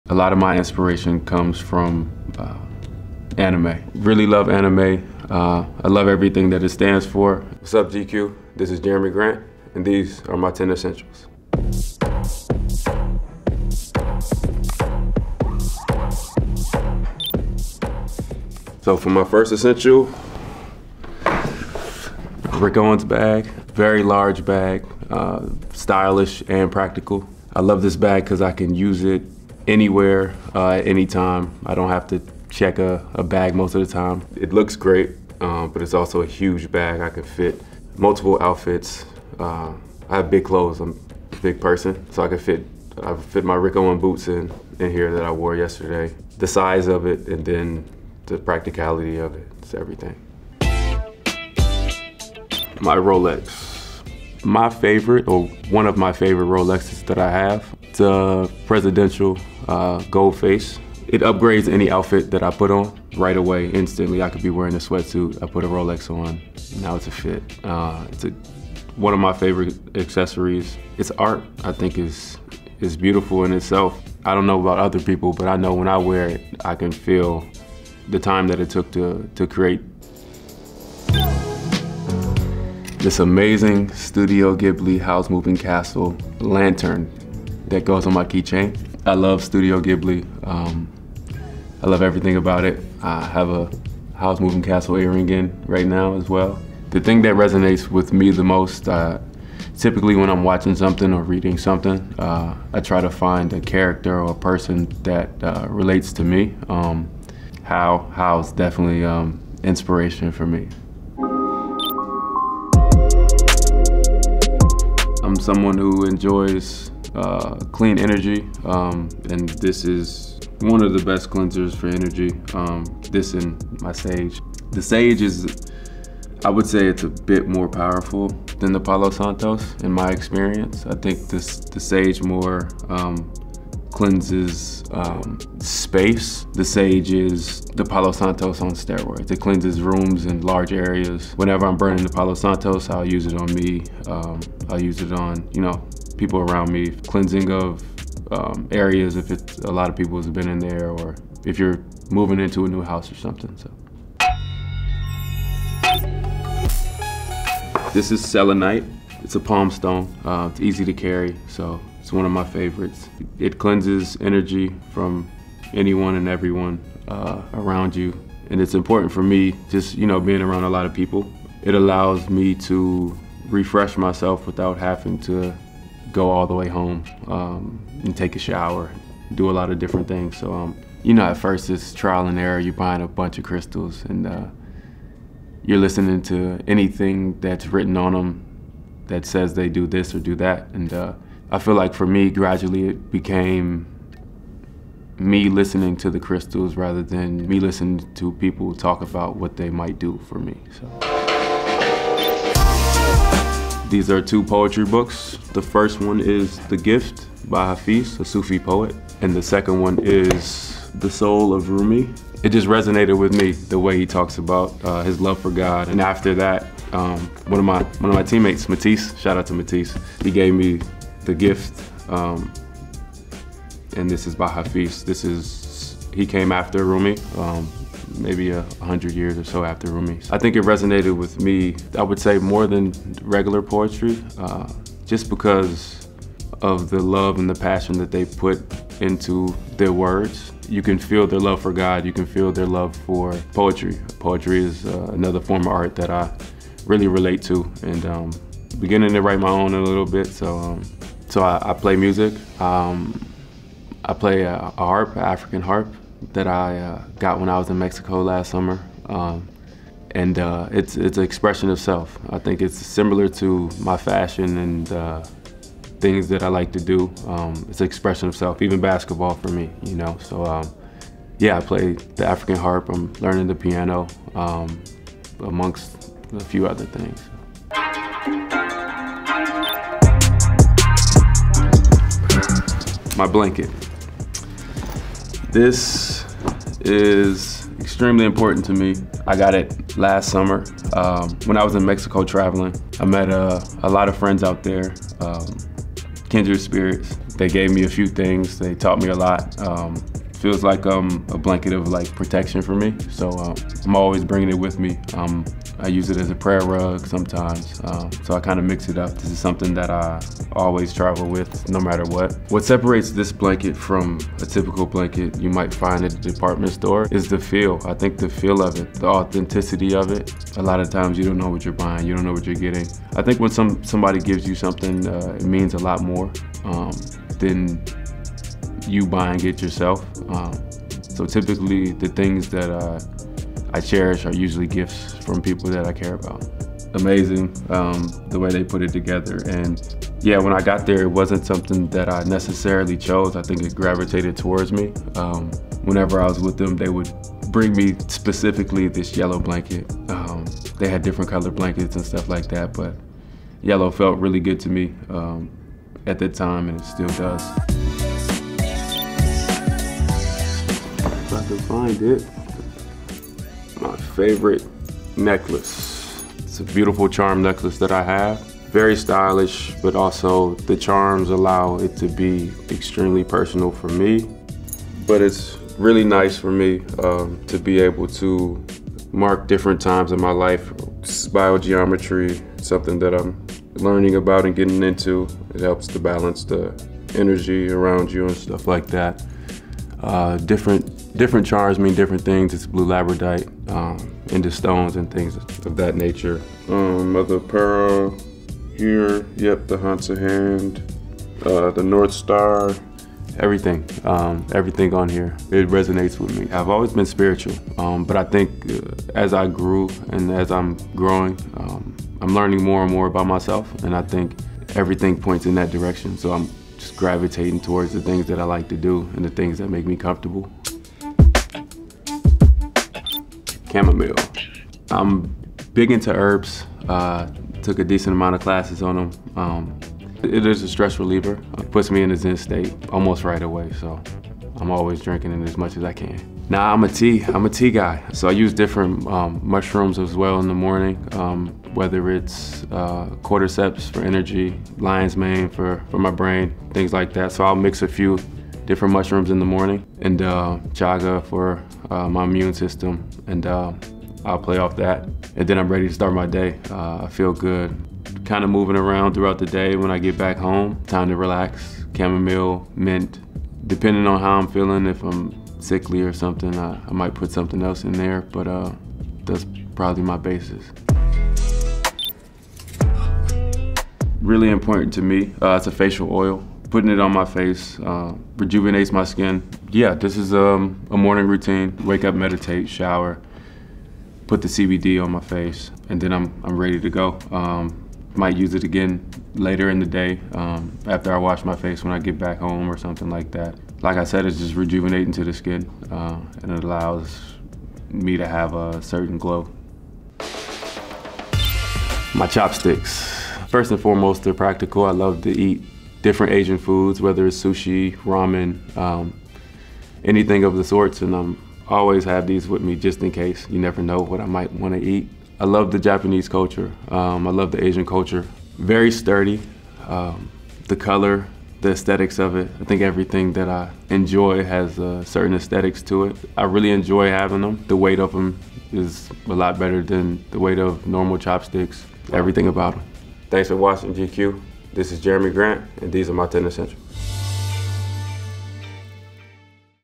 10 Things Portland Trail Blazer Jerami Grant Can't Live Without | GQ Sports
"A lot of my inspiration comes from anime." From his Rick Owens travel bag and a Presidential Rolex to some palo santo and a special blanket from Mexico, these are Portland Trail Blazers' Jerami Grant's ten essentials.